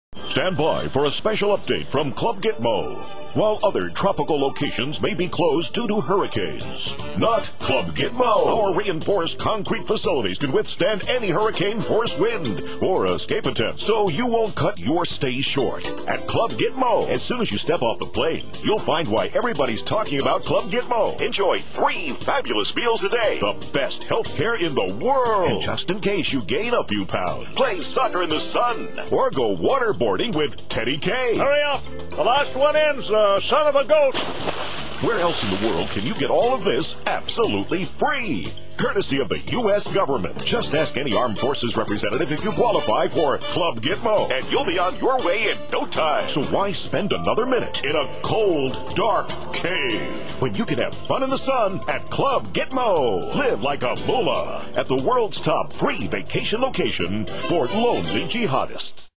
Club Gitmo Commercial
Based on Rush Limbaugh's parody ad, some pics from his site and actual pics from Guantanamo Bay.